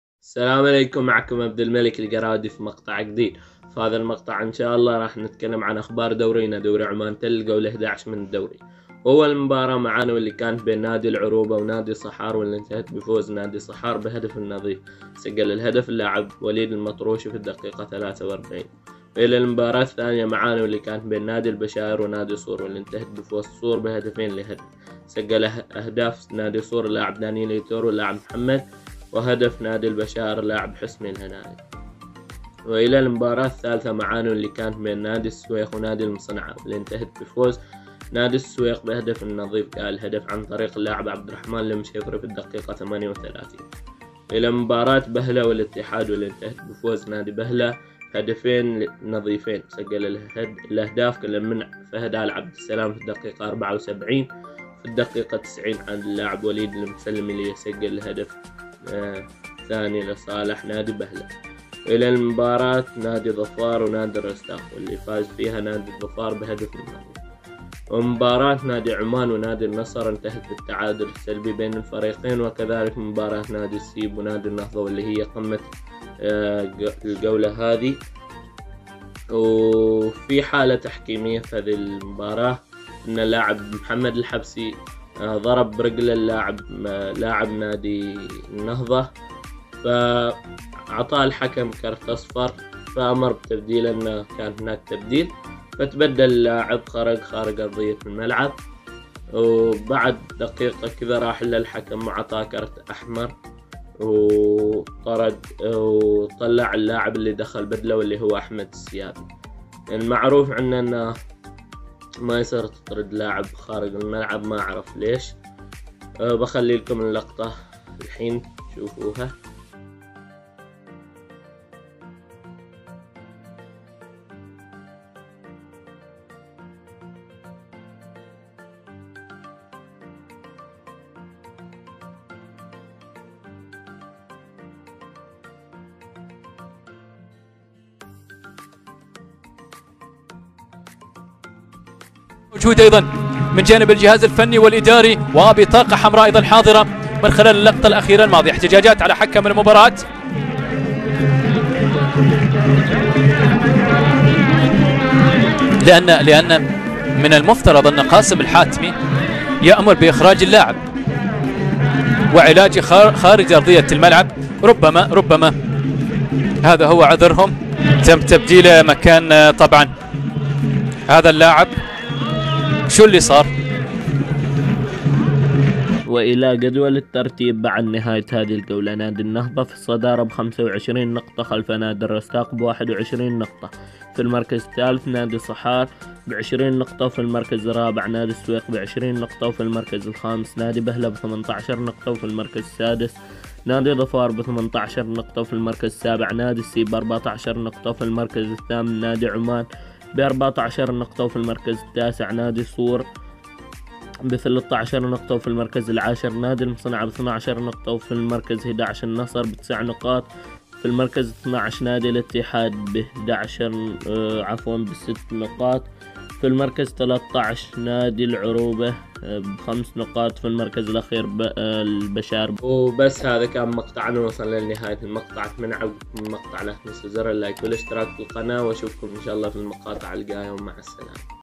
حاله تحكيميه مثيره للجدل في قمه مباريات الجوله|أخبار الجوله ال١١ من دوري عمانتل
معكم عبدالملك الجرادي يوتيبرز عماني أقدم أخبار دوري عمانتل واخبار المنتخب العماني
اتمنى دعمكم
الحلم100k🔥